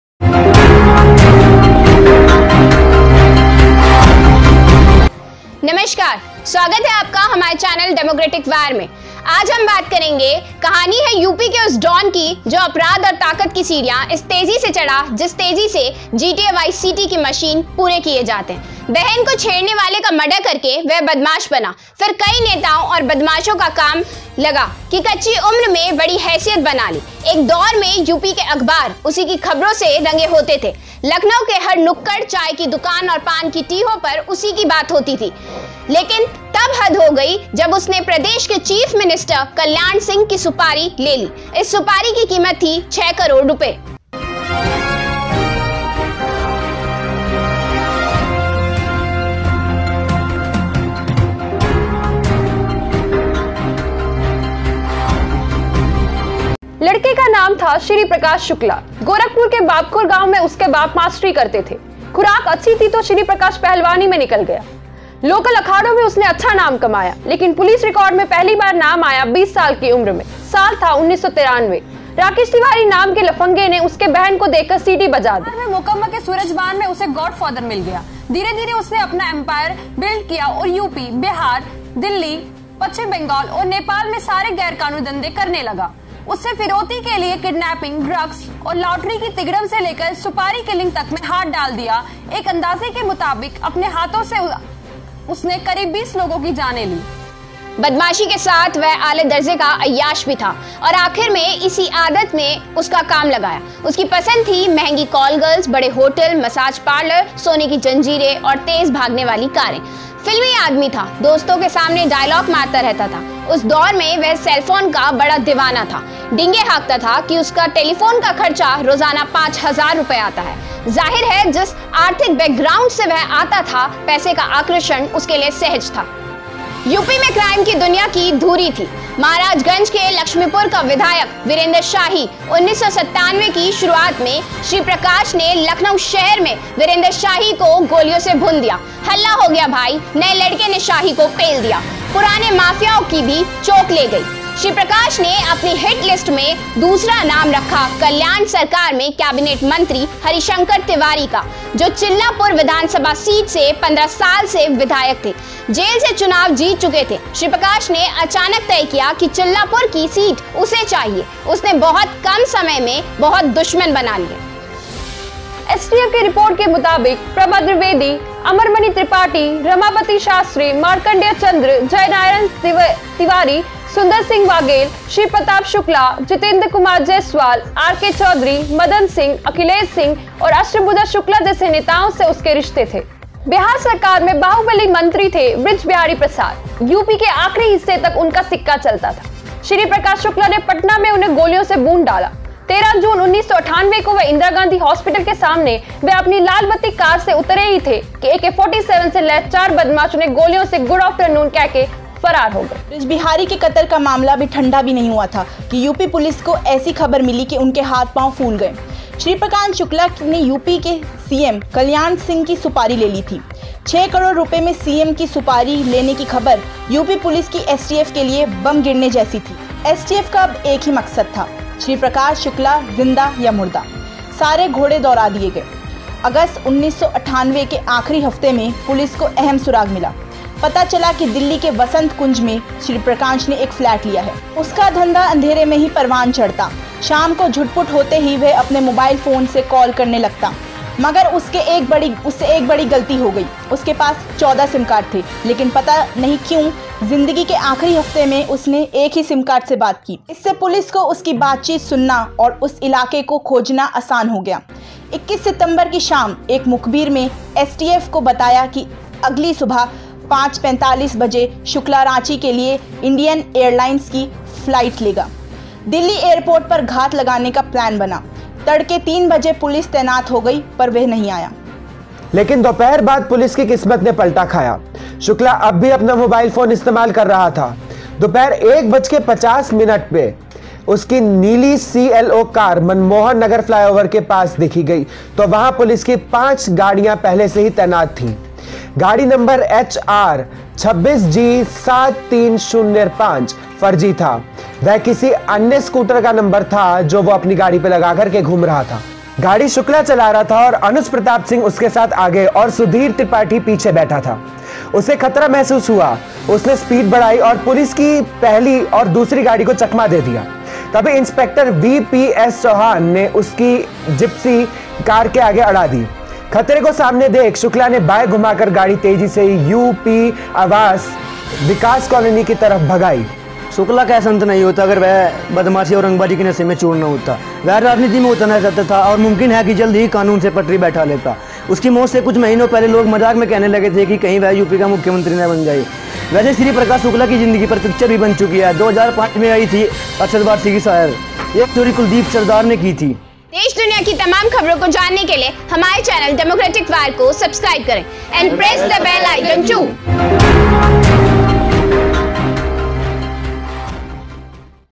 यू.पी का वो 25 साल का डॉन,जिसने मुख्यमंत्री की ली थी सुपारी 6 करोड़ रुपये में
इसी डॉन के आतंक की वजह से किया गया था एसटीएफ के गठन